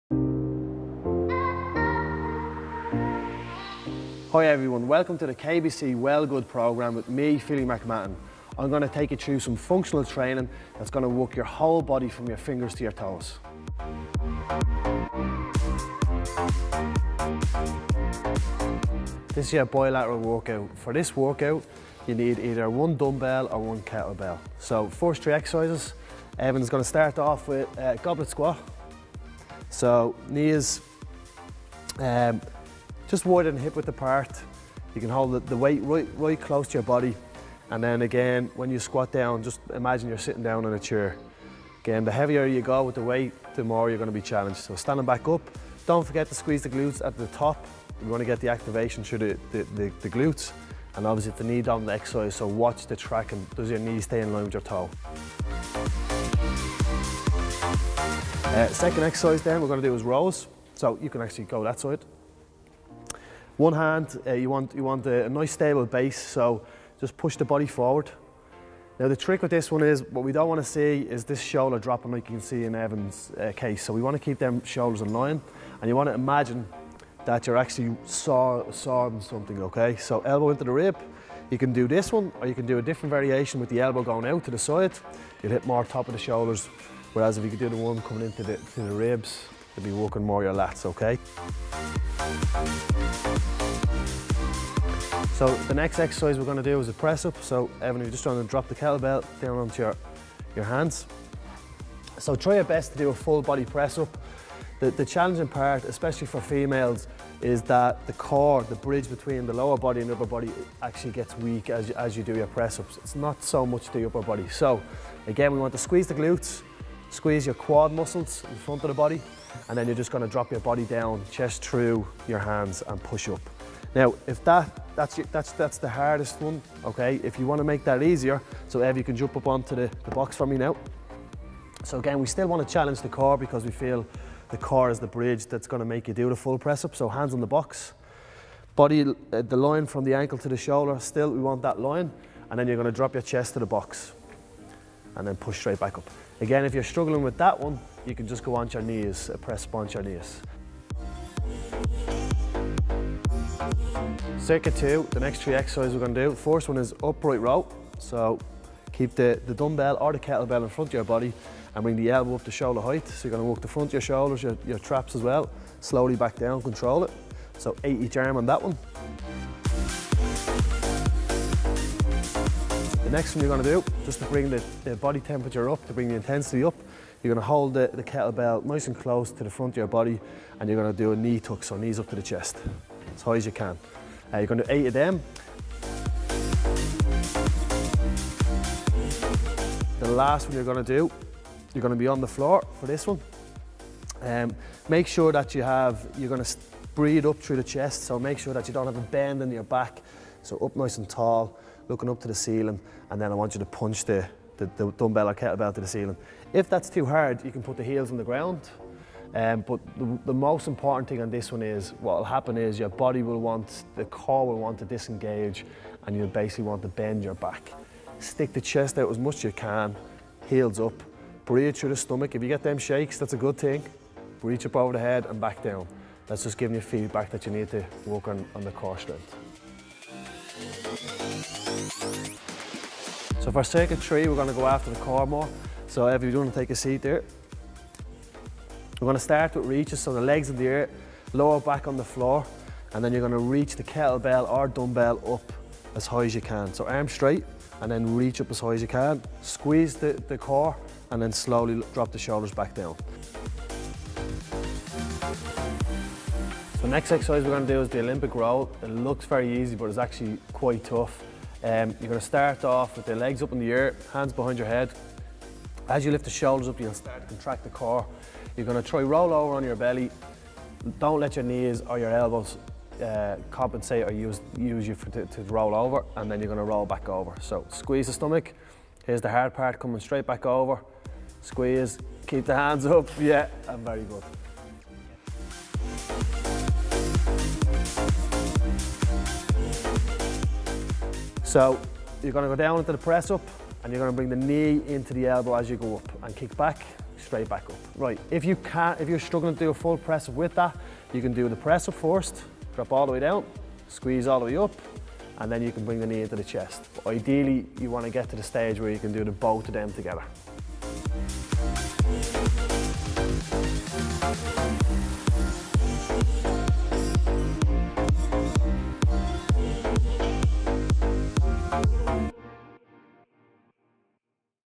Philly McMahon's Bilateral Workout - WellGood
Philly McMahon is getting us moving with the second fitness challenge of KBC WellGood.

Give this a go for an full body workout. You’ll need one dumbbell or kettlebell plus a box, bench or chair.

8 reps per exercise and 3 rounds of each circuit

Circuit 1
- Goblet squat
- Landmine rows
- Press ups

Circuit 2
- Upright row
- Knee tuck
- Hollow press

Circuit 3
- Reaches
- Olympic rolls
- Spiderman press ups